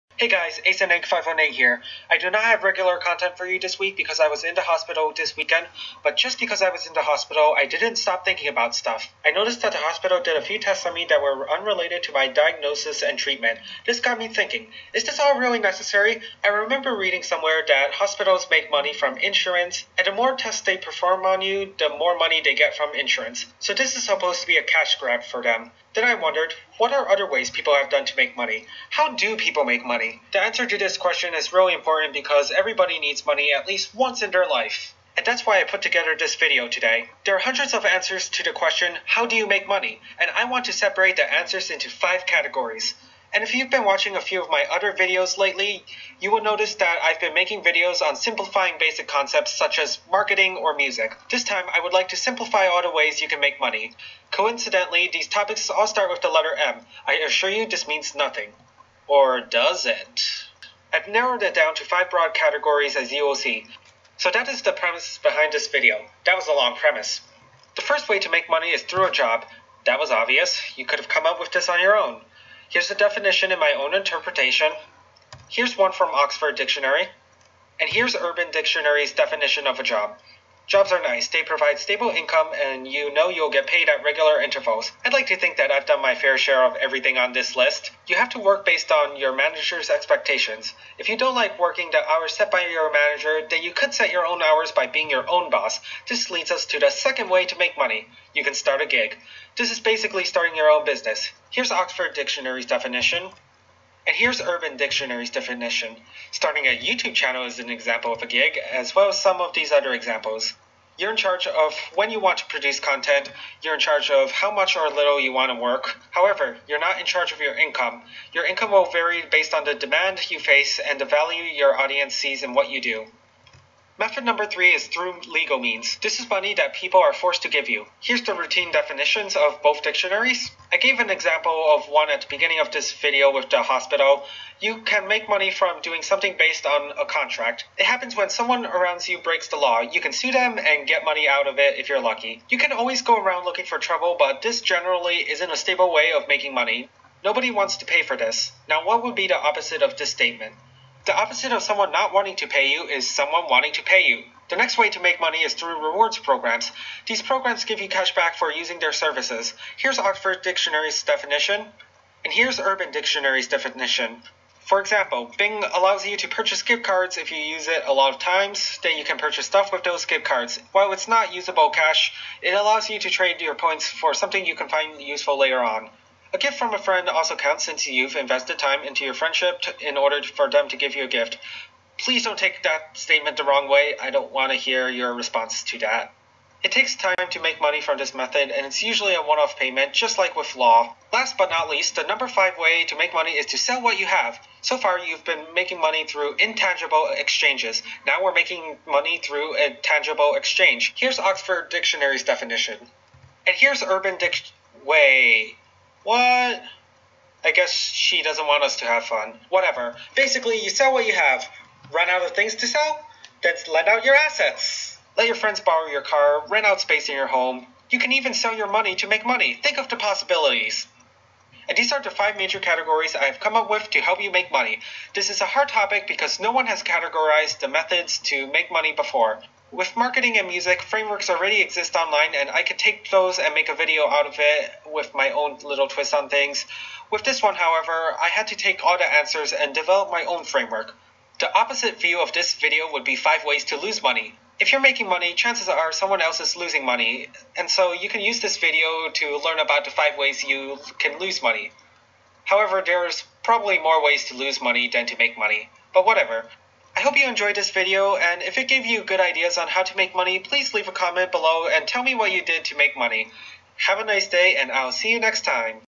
How to Make Money
This video categorizes the many ways to make money in this world. You’re given many advice when you look up the answer to this question, and I attempt to condense every advice to 5 basic concepts. This is the third video in my series of condensing complex ideas into manageable bits.

Here are the 5 ways to make money discussed in the video:
1. Job
2. Gig
3. Law
4. Reward
5. Sell

Anything else you come up with can probably fall under one of these categories.

I did not intend to make this video. I was in the hospital this weekend and I couldn’t make the video I had originally planned. Next week however, I’ll have no excuses. Sorry about the delay of my video. Then again, I haven’t released the topic of the video I wanted to publish, so you don’t know what it is yet :)